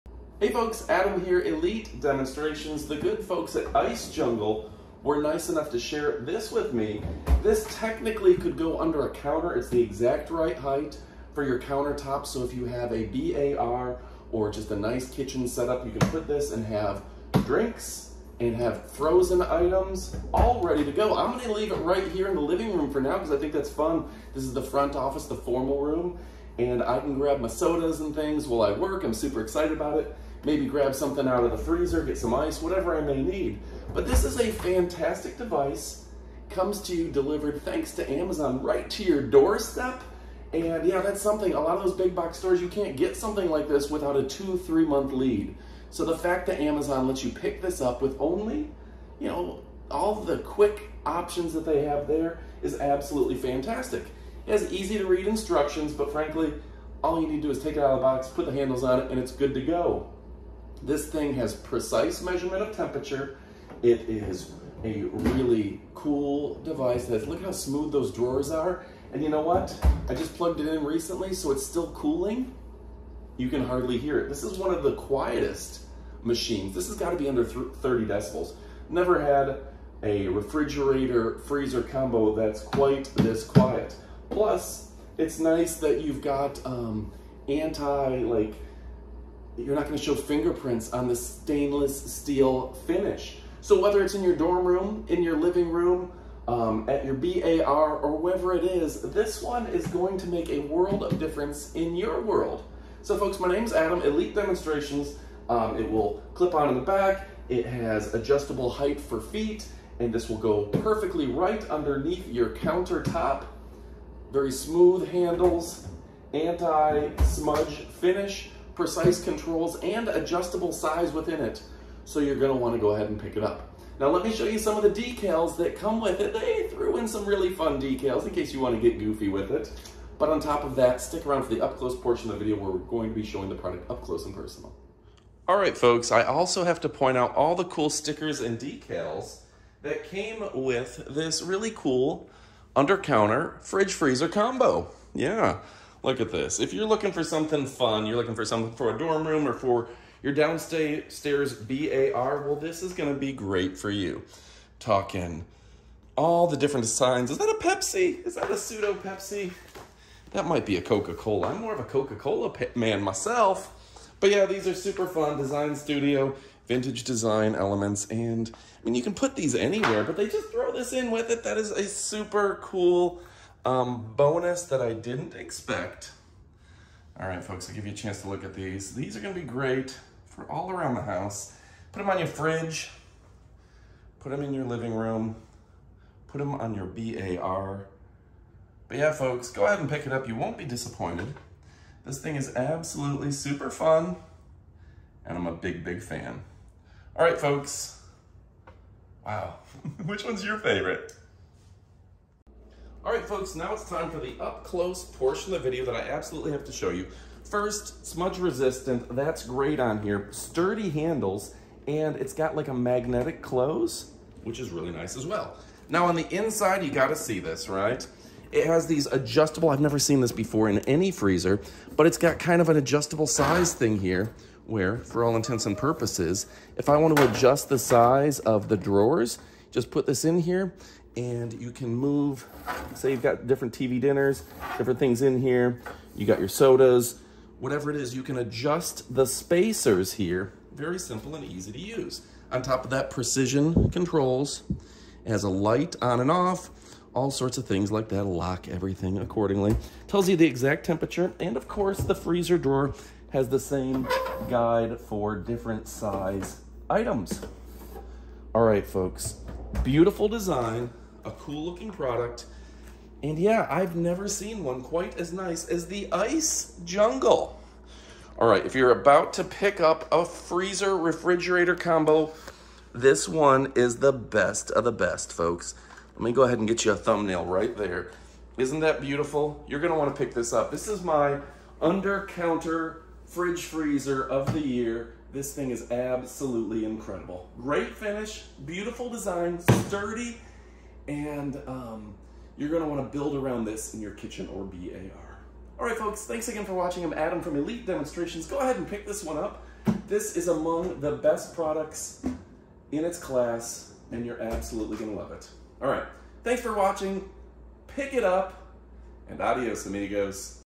IceJungle Under Counter Refrigerator for Home Use
✔️[DIGITAL TOUCH LED DISPLAY °C /°F]: Adjustable temperature LED display screen range from 0℃-20℃/32℉-72℉, Ensure that the food and beverages in the refrigerator do not deteriorate.
    ✔️【PRECISE TEMPERATURE CONTROL】Our refrigerator is equipped with electronic temperature controller, which can accurately control the temperature of each degree (precision temp. Control), ensuring the quality of your beverages.
    ✔️[STAINLESS STEEL SLIDE-OUT DOUBLE DRAWER WITH DIVIDER]: The Fridge drawer equipped with Double Drawer and fits more than the traditional side by side refrigerator It has a divider in the main Fridge section and also has a sliding Fridge drawer to separate foods to help you better plan the internal space of the refrigerator
    ✔️[SMALL REFRIGERATOR, LARGE CAPACITY]: 23.4" x 23.03” x 32.28"(w x d x h) can store 142 standard 12 oz. cans. Super ideal for beer soda at home, office, dormitory& bar
✔️[AUTOMATIC LIGHT ON ANN WARRANTY SERVICE]: Automatic door closing function, Turn on the light when you open the door, Open door alarm after 60s, High and low temperature alarm.We provide a one-year warranty service.

Follow us on Amazon